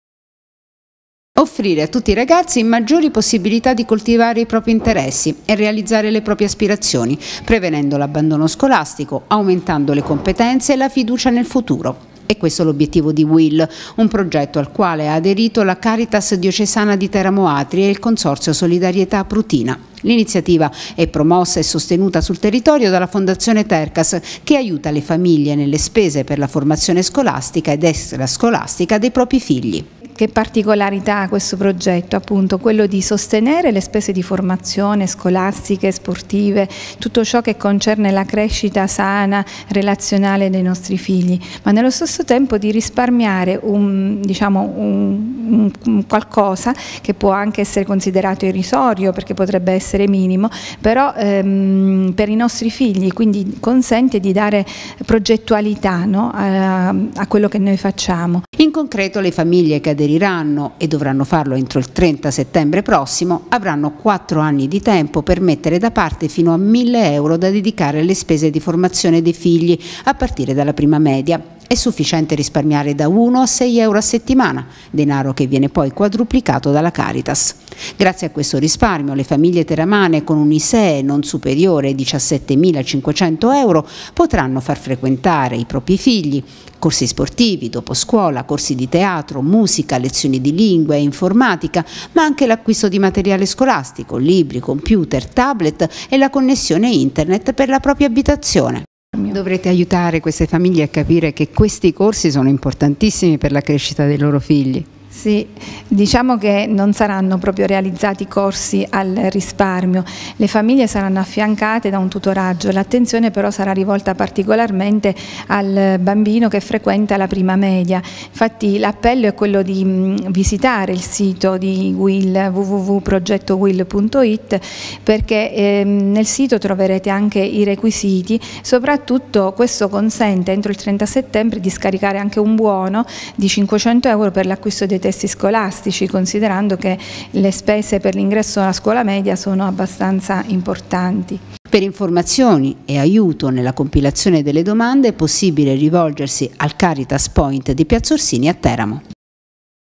Caritas e Fondazione Tercas contro l'abbandono scolastico
La Caritas Diocesana di Teramo e la Fondazione Tercas hanno aderito al progetto Will contro l'abbandono scolastico e la povertà educativa. Grazie a questa iniziativa le famiglie più bisognose potranno accedere ad un aiuto economico per la formazione scolastica ed extra scolastica dei propri figli